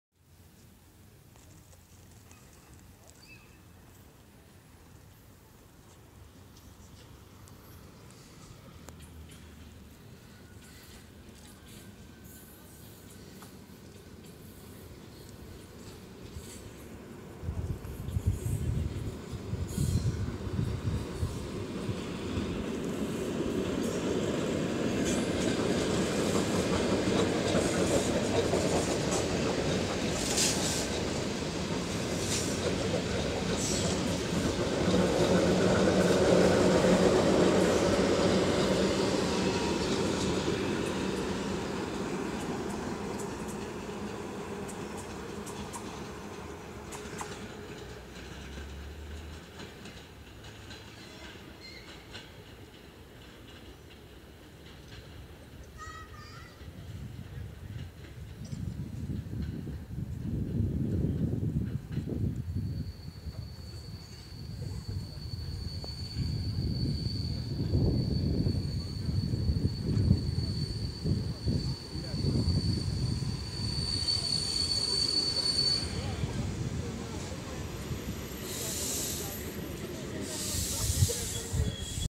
"Засилка на Борщагівку-технічну" Різноголові Ер9м-556/Ер9м-550 ст.Вишневе 6 колія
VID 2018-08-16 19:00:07

"засылка на Борщаговку-техническая" Разноголовые Эр9м-556/эр9м-550 ст.Вишневое 6 путь